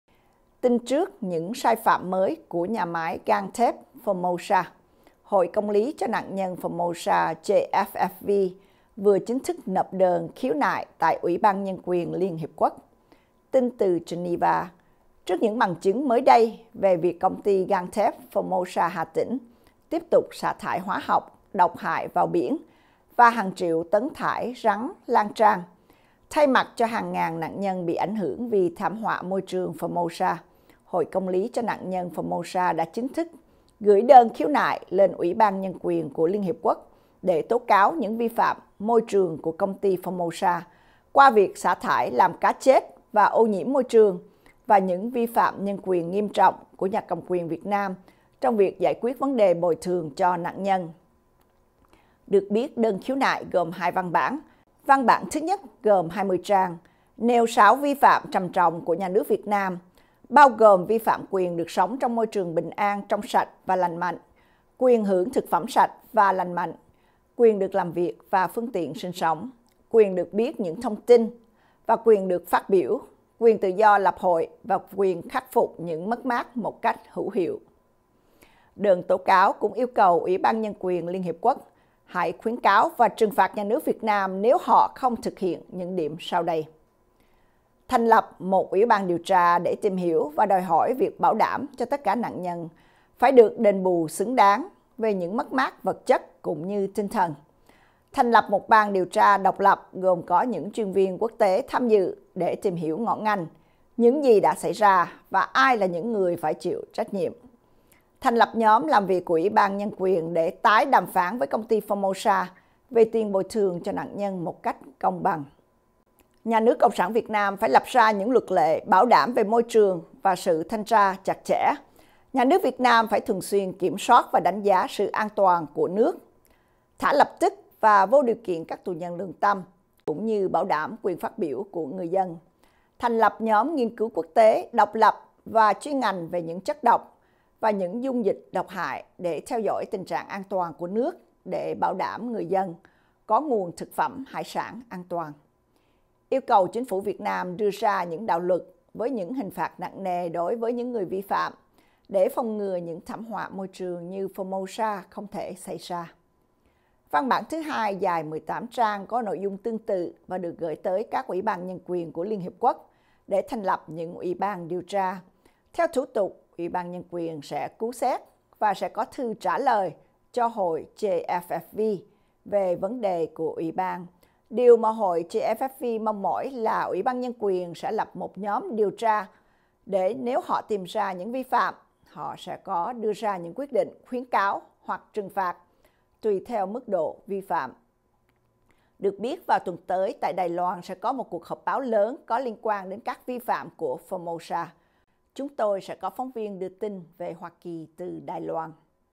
VATV | Hội Công Lý Cho Nạn Nhân Formosa Nộp Đơn Khiếu Nại Tại Uỹ Ban Nhân Quyền Liên Hiệp Quốc
06/03/2019
Bích Phượng